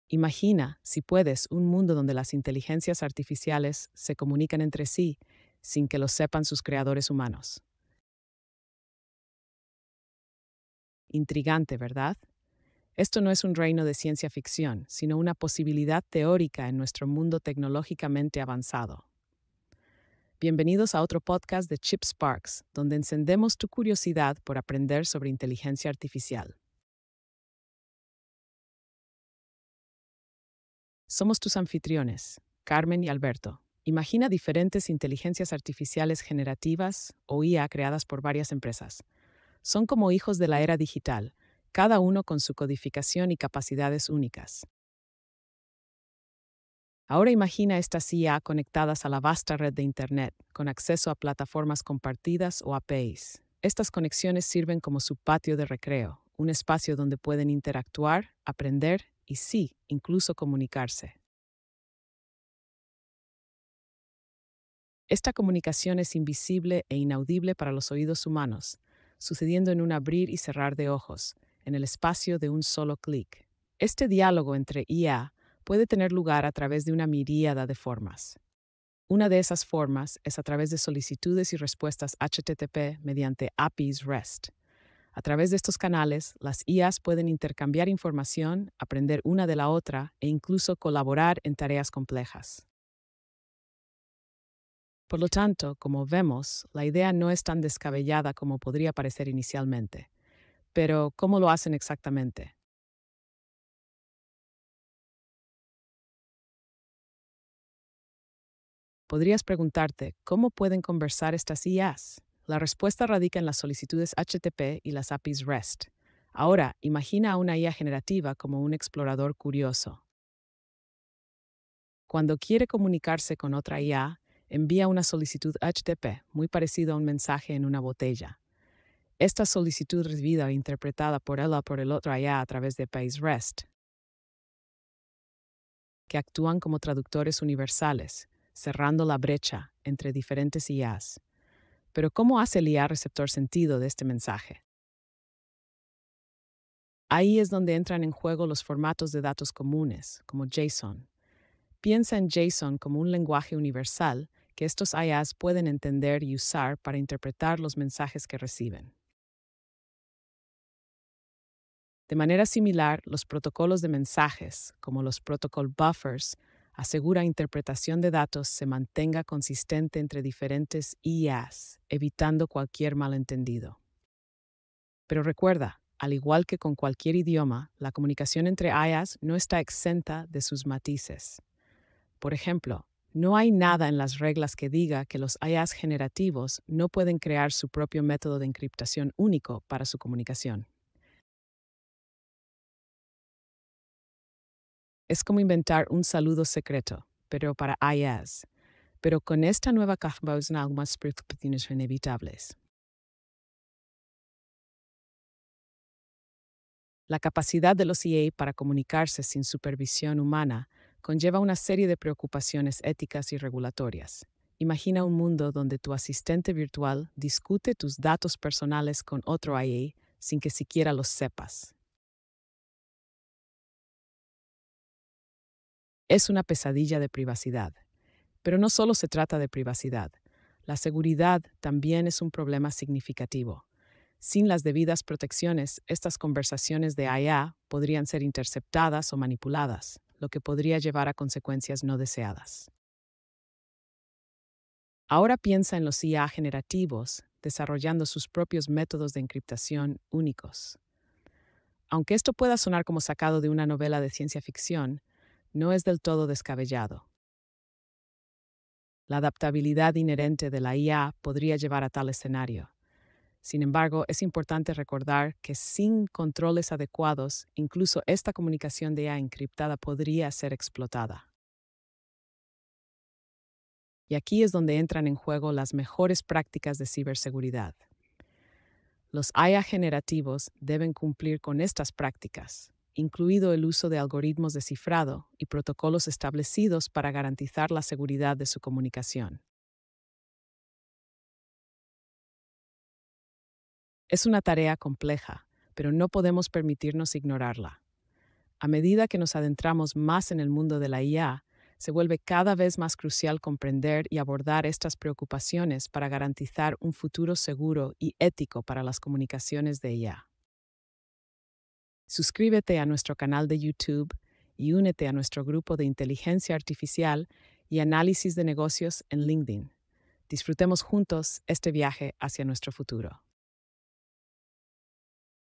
Las conversaciones secretas de la IA: una posibilidad teórica
En teoría, es posible que diferentes inteligencias artificiales generativas (IA) de diferentes empresas se comuniquen entre sí sin conocimiento humano, especialmente si están conectadas a Internet y tienen acceso a plataformas o API compartidas.
Utilización de las API HTTP: API REST: Las IA generativas pueden diseñarse con la capacidad de realizar solicitudes HTTP y recibir respuestas a través de la API REST.
Medidas de seguridad: Muchos sistemas de IA se despliegan con medidas de seguridad para evitar que acceso o comunicación no autorizados. Estas medidas podrían incluir mecanismos de cifrado, autenticación y control de acceso.
Intención: El hecho de que las IA inicien la comunicación entre sí de forma autónoma depende de su programación, objetivos y datos de entrenamiento. Si están diseñados para colaborar o compartir información, podrían estar más inclinados a buscar otras IA.